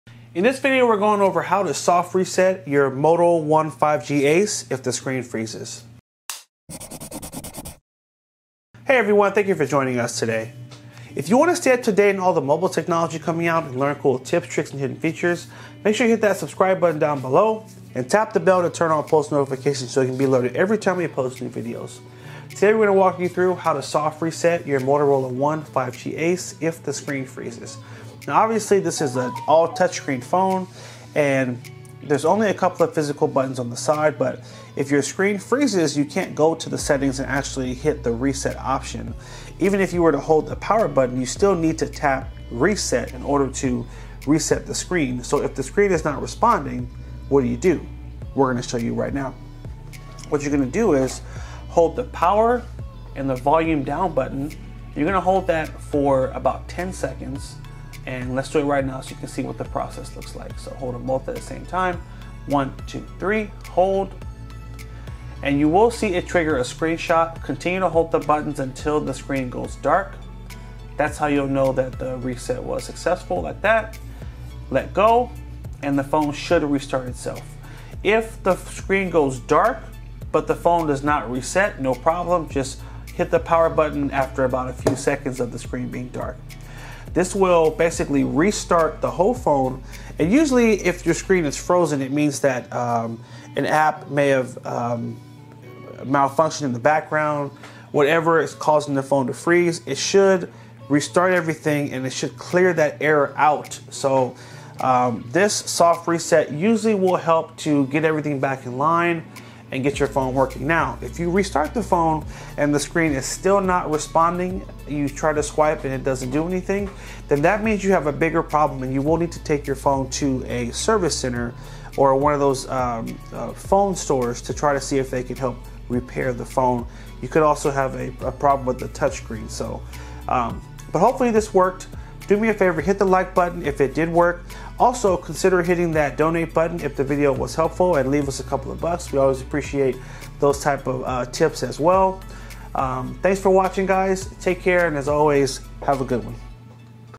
Moto One 5G Ace How to Soft Reset If the Screen Freezes
Walk through tutorial on how to soft reset your Moto One 5G Ace if the screen is stuck, frozen, or unresponsive. The steps displayed in the video will help you restart your phone using the physical buttons.

💡You need one of these for your Moto One 5G Ace